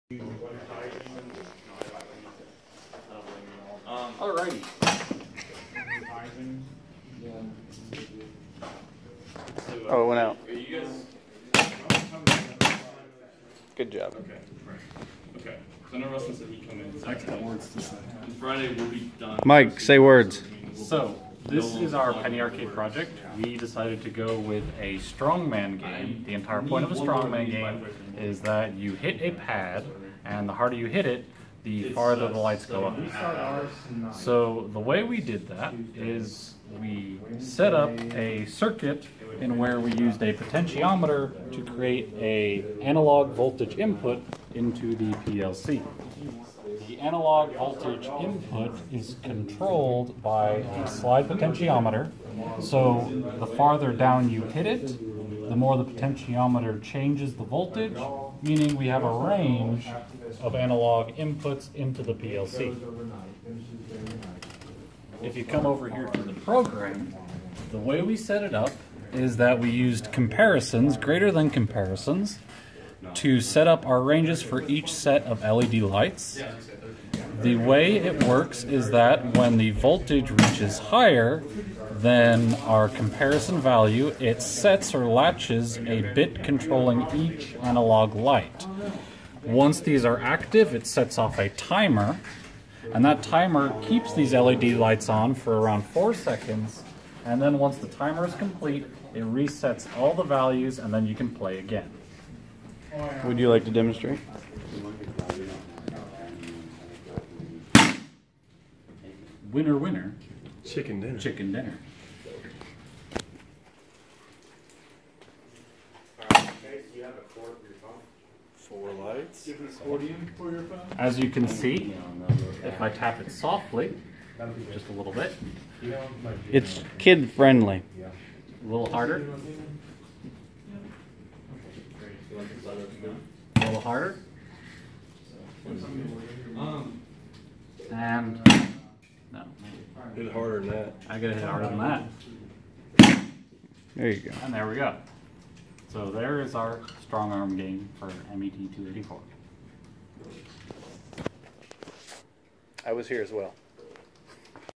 Strong arm penny arcade game - PLC -MET284
Shows our project and how it works with the programming behind it.